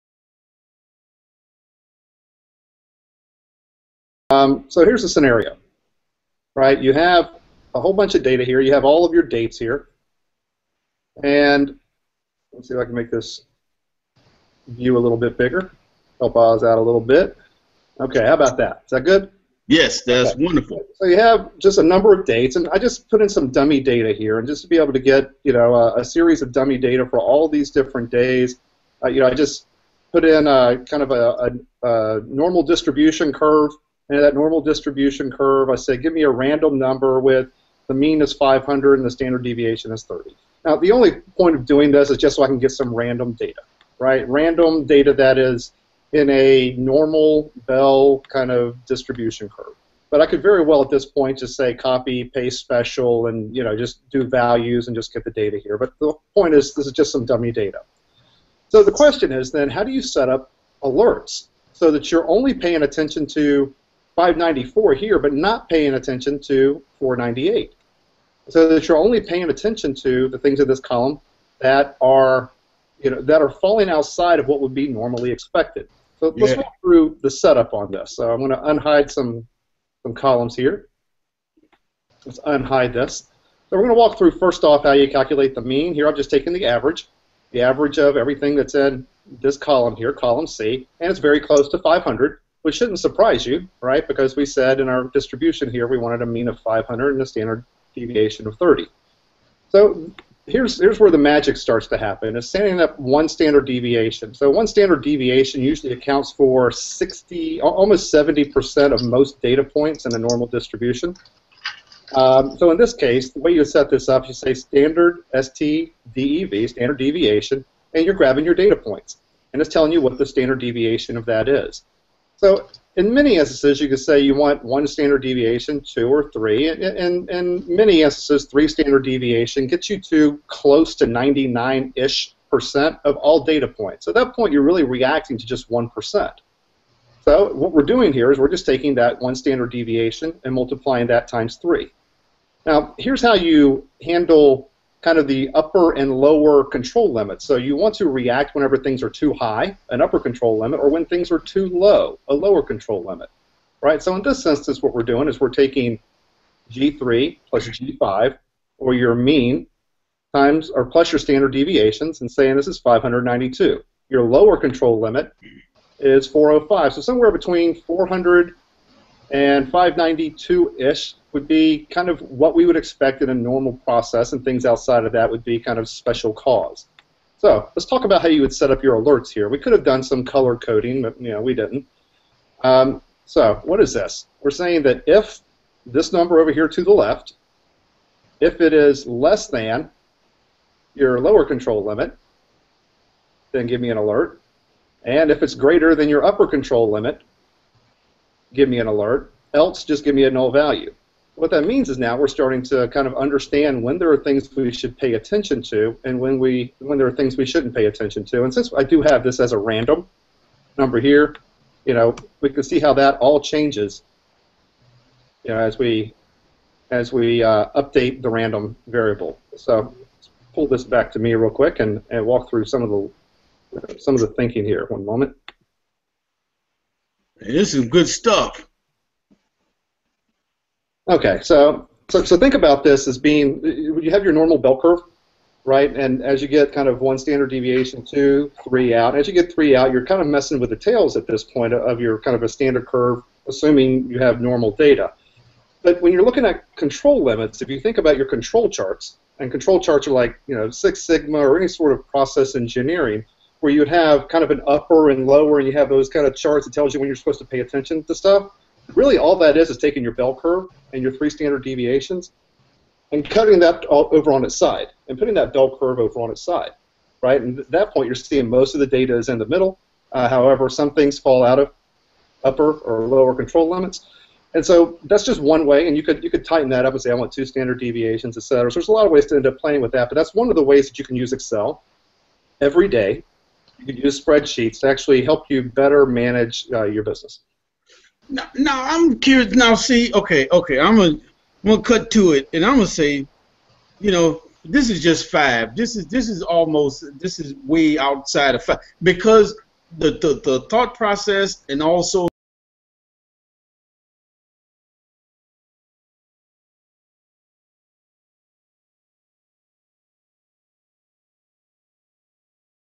How To Do Standard Deviation in Excel
Using Standard Deviations in Excel. Rick explains how to use the man (average) and standard deviations to determine whether or not your process is "In Control"

Google Plus:
Oz Du Soleil: